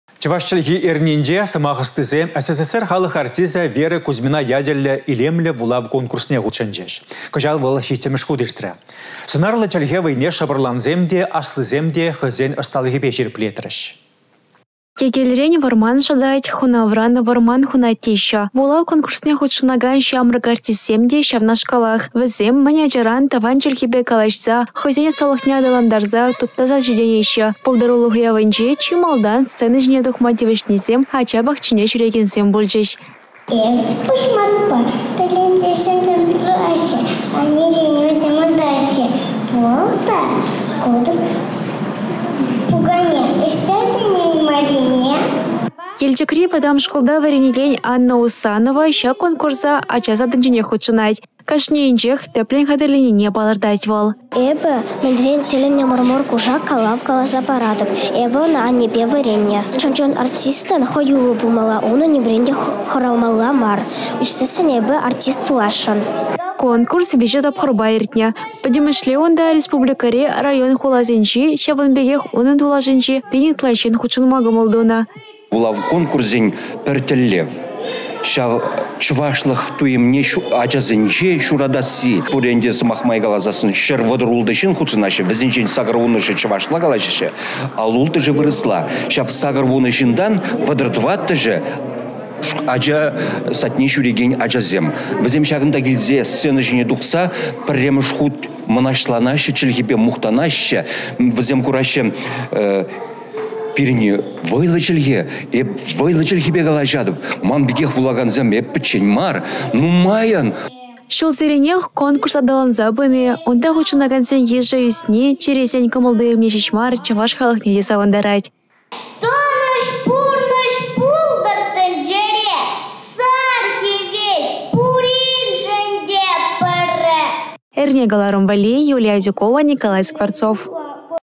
Илемлĕ вулав конкурсĕ пултаруллисене палăртрĕ
Чăваш чĕлхи эрнинче сăмах ăстисем СССР халăх артисчĕ  Вера Кузьмина ячĕллĕ илемлĕ вулав конкурсне хутшăнчĕç. Кăçал вăл 7-мĕш хут иртрĕ. Сăнрлă чĕлхе вăйне  шăпăрлансем те, аслисем те хăйсен ăсталăхĕпе çирĕплетрĕç.